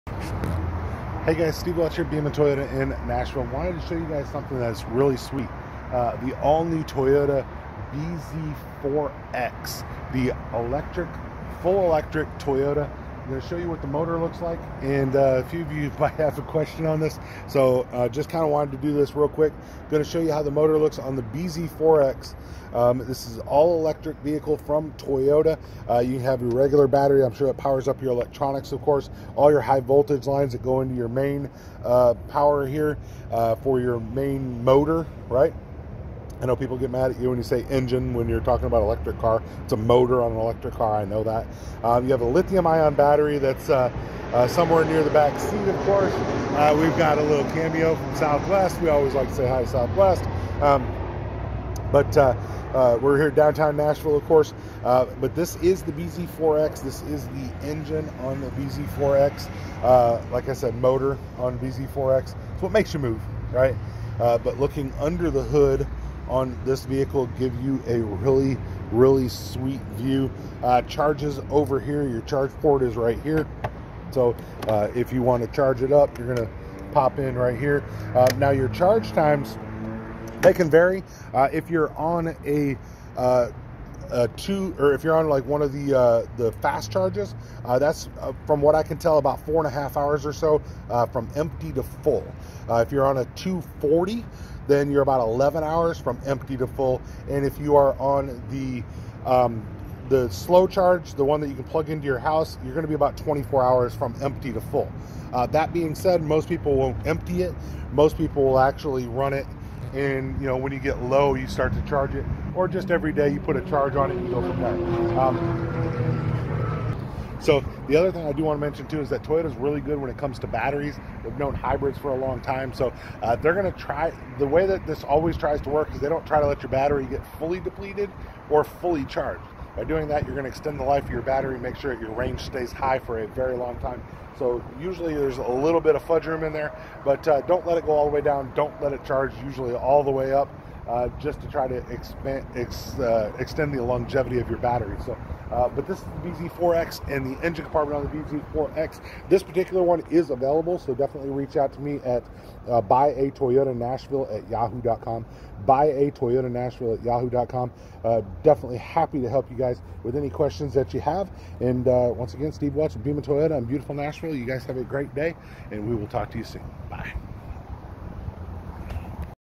A look inside the engine compartment of the Toyota BZ4x
The motor of the Bz4x is definitely different than other engines that Toyota has. Take a look.